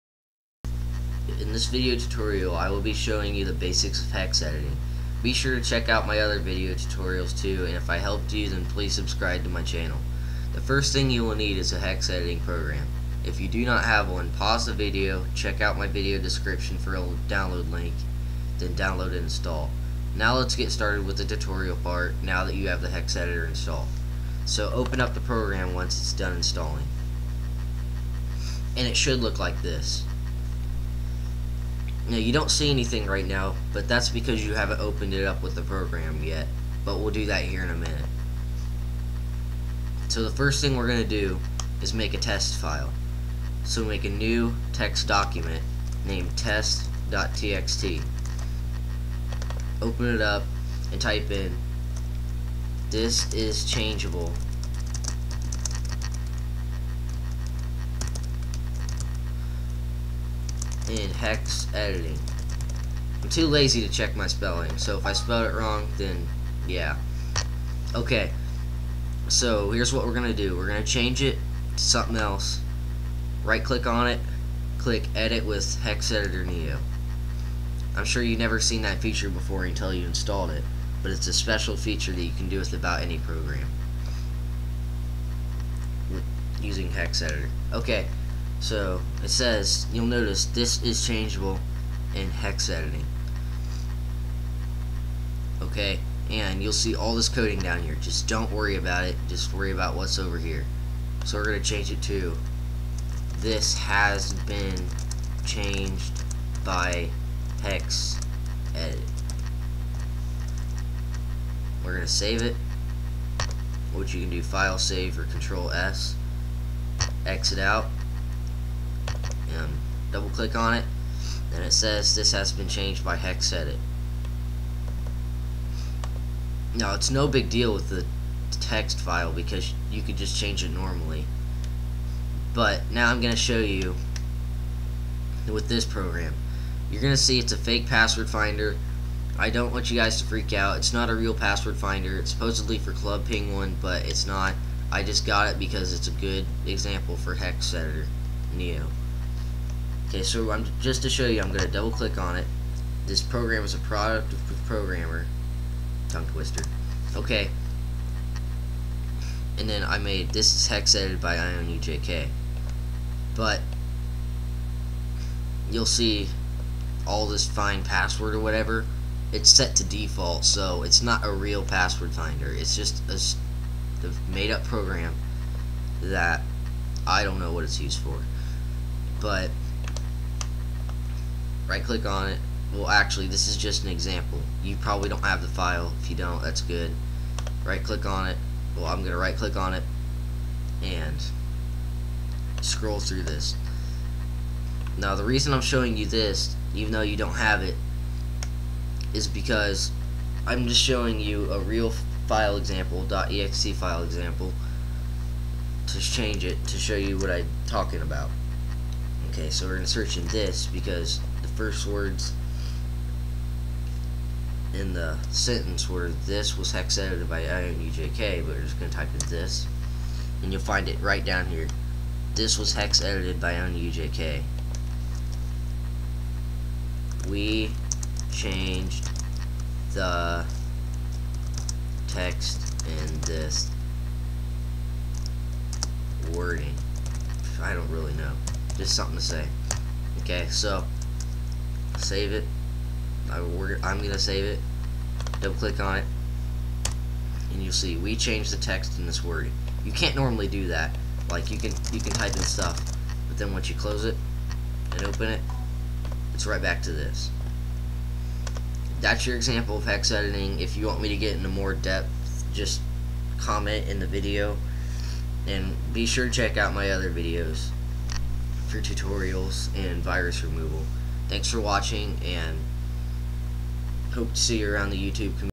Hex editing Tutorial
This video will show you the basics of hex editing.